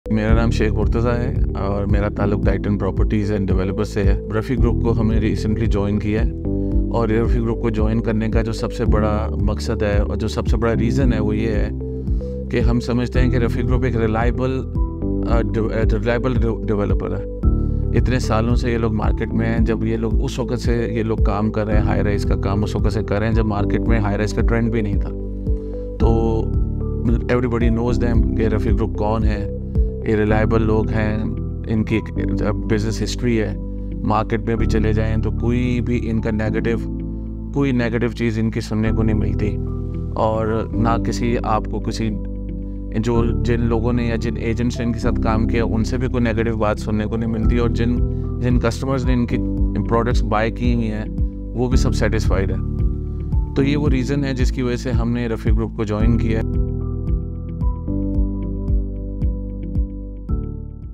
Rafi Group Welcomes TITAN Properties on Board!
Rafi Group welcomes TITAN Properties on joining forces with Rafi Group
as an official sales partner for Green Palms Lahore!